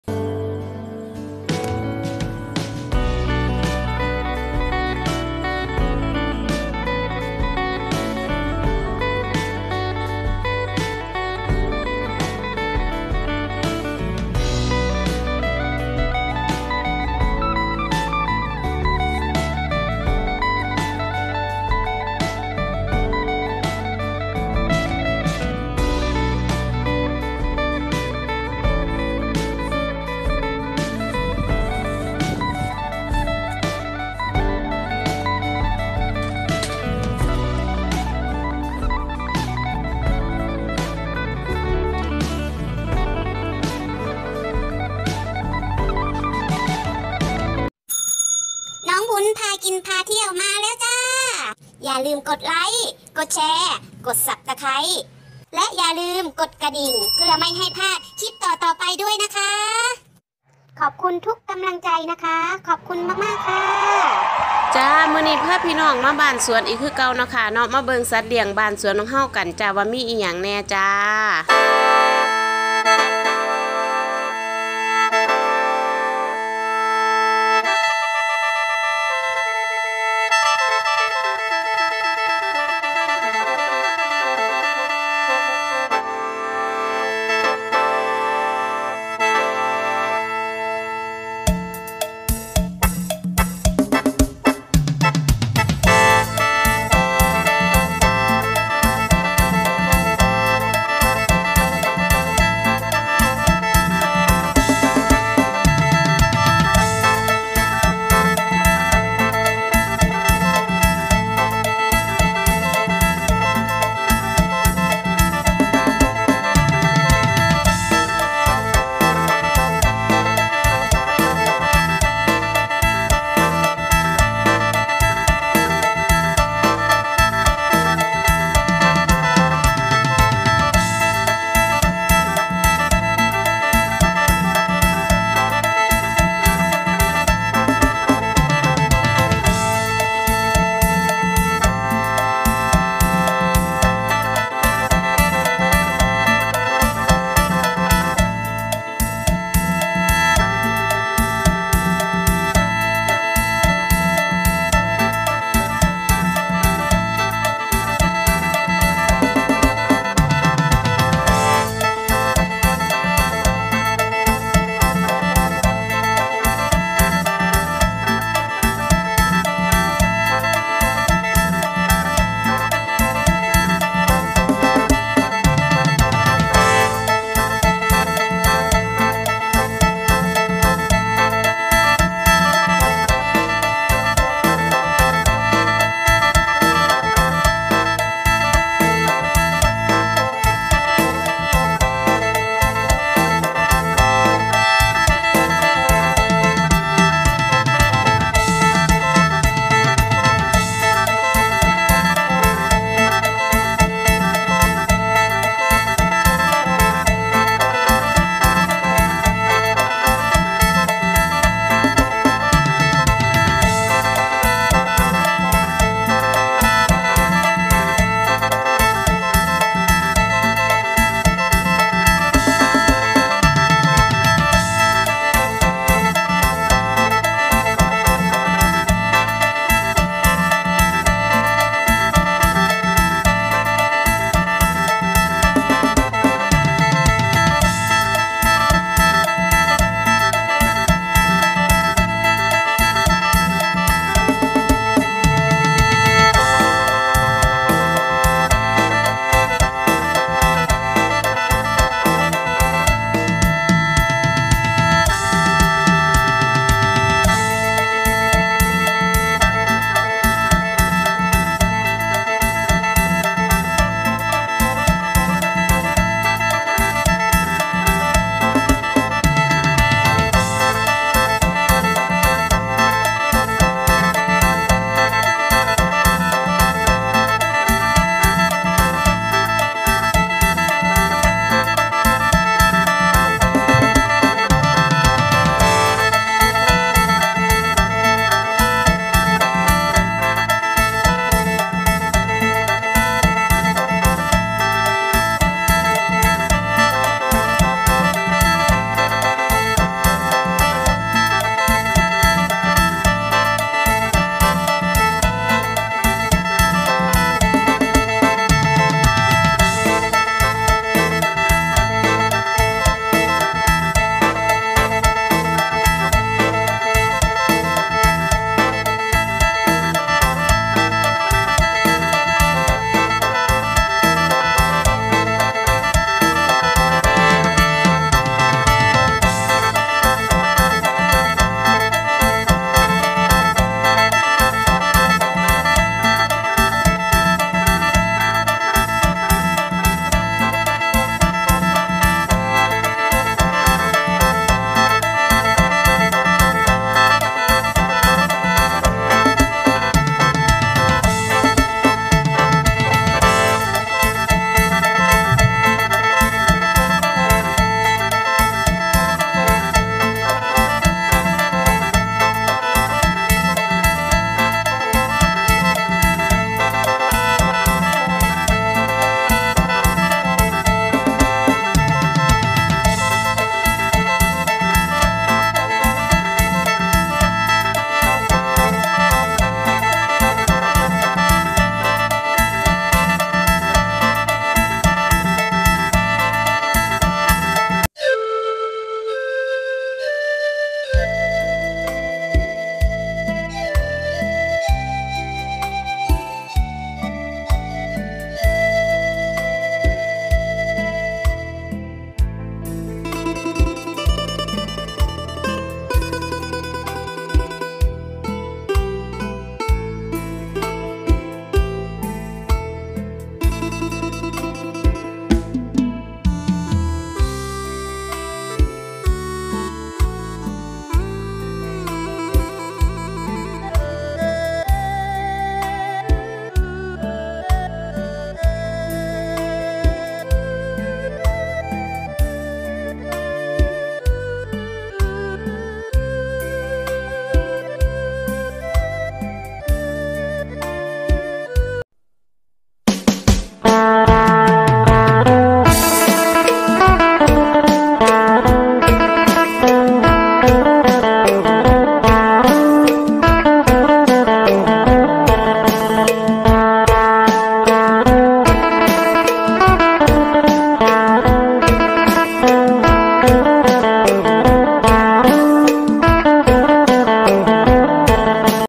Ep.2 ให้อาหารสัตว์เลี้ยงที่บ้านสวน บรรยากาศยามแลง คลิปส่งมาจากน้องวุ้นจ้า @น้องวุ้นพากินพาเที่ยว
ให้อาหารเป็ดอาหารไก่ที่บ้านสวน บรรยากาศยามเย็น บรรยากาศทุ่งนา ชีวิตบ้านสวน @น้องวุ้นพากินพาเที่ยว